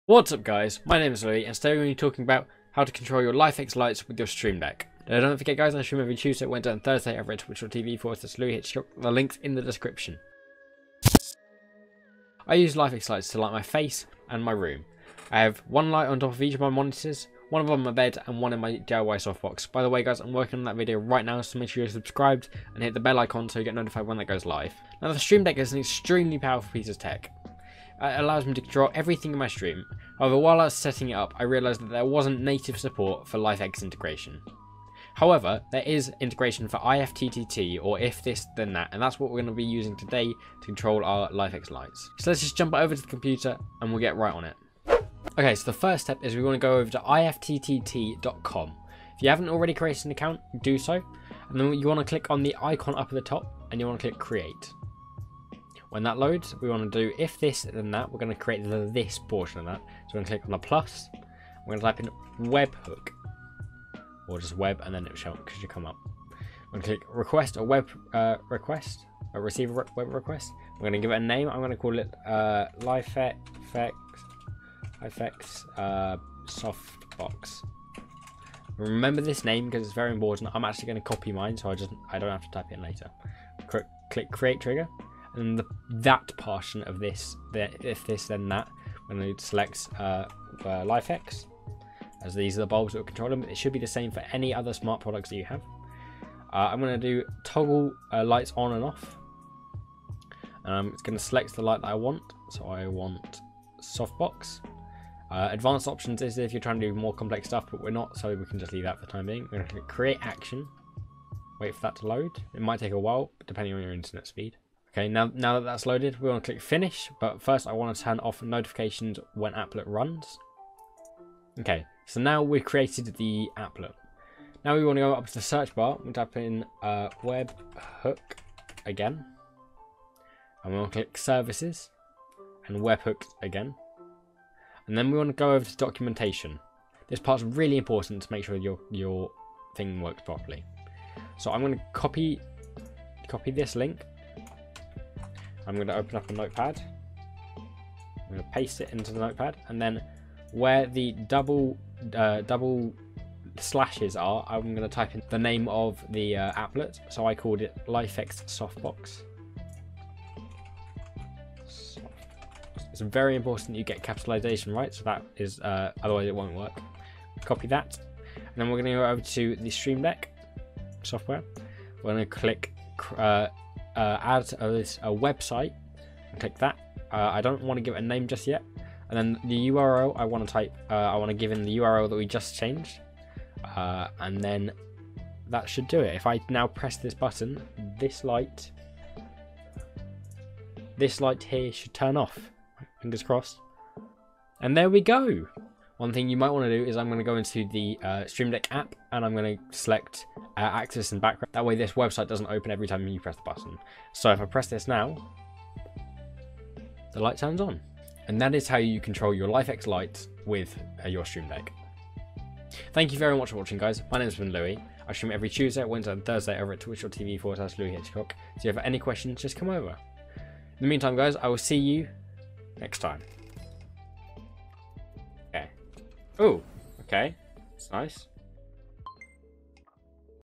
How to Control YOUR LIFX lights with a StreamDeck!
In this video I help you connect your LIFX Lights with your Elgato StreamDeck!

Description tags: Philips, Elgato, Lights, Smart, Home, LIFX, Setup